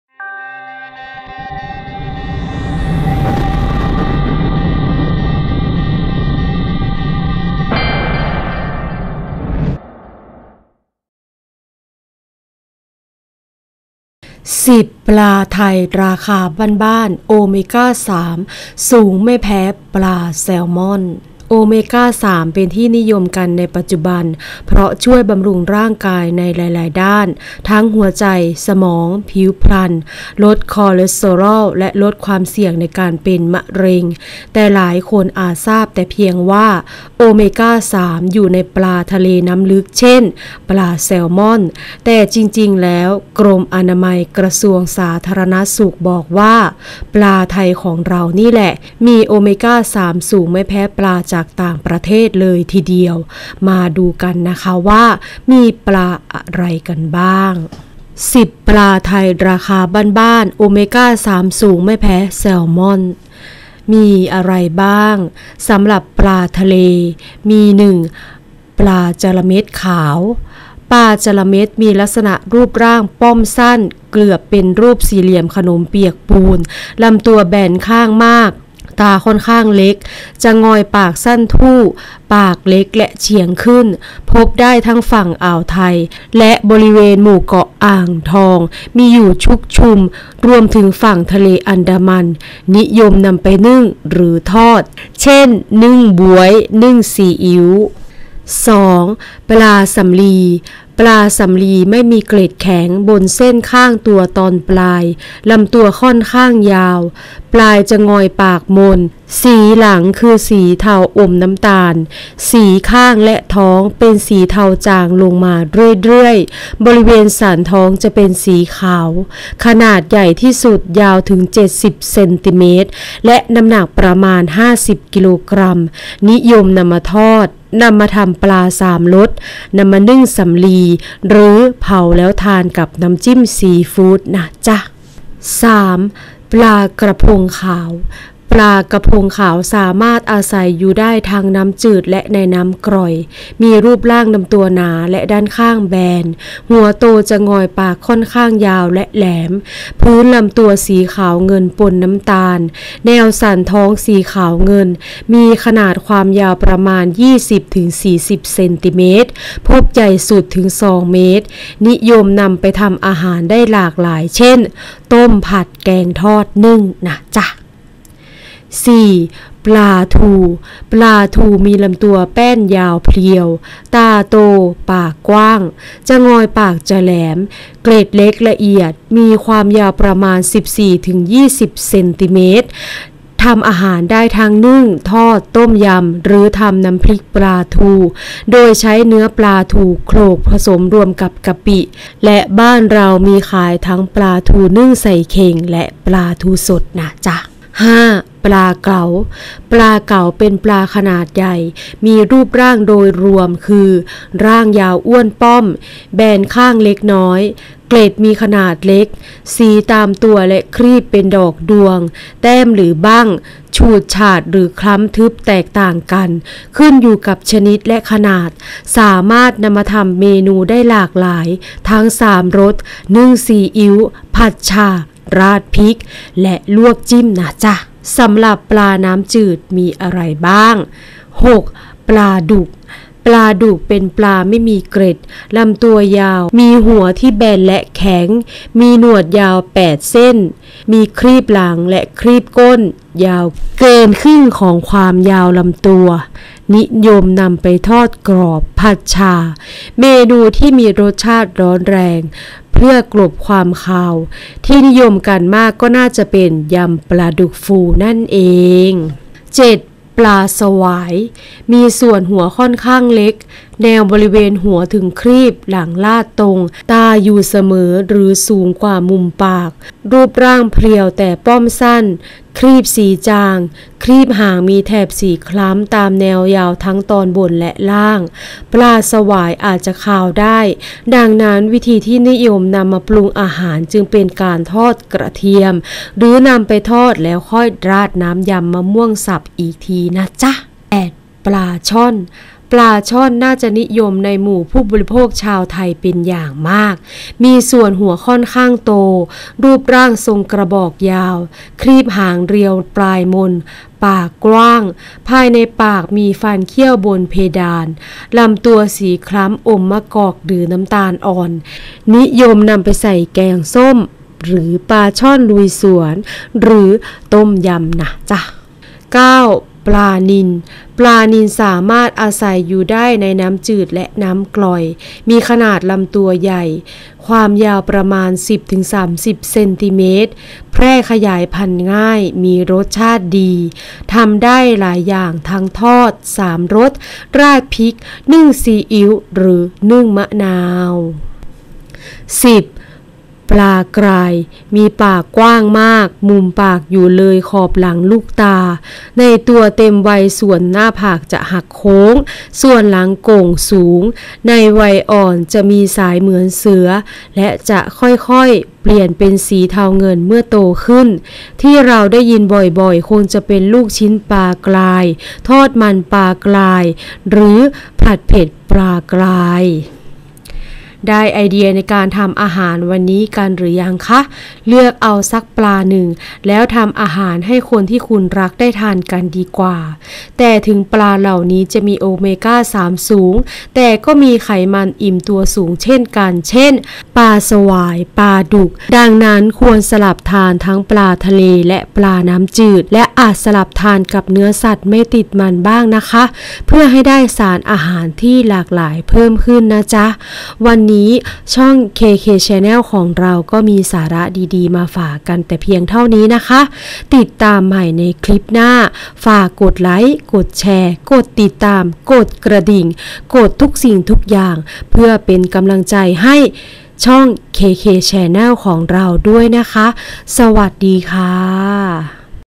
10 ปลาไทยราคาบ้านๆ!!!!โอเมก้า 3 สูงไม่แพ้แซลมอน มาดูกันว่ามีปลาอะไรกันบ้าง
10 ปลาไทยราคาบ้านๆ!โอเมก้า 3 สูงไม่แพ้แซลมอน มาดูกันว่ามีปลาอะไรกันบ้าง